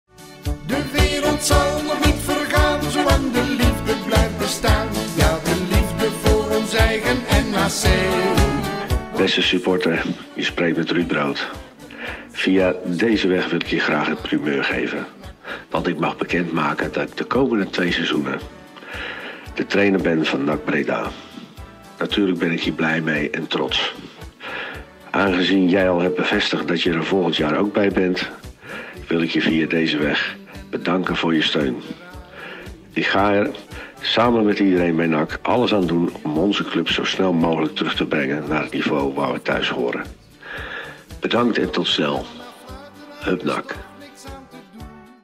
Ruud Brood belt supporters met een primeur! 💛🖤
Ruud Brood is ook de komende twee seizoenen de hoofdtrainer van NAC Breda. De trainer belde woensdag zelf de seizoenkaarthouders om het nieuws te verkondigen.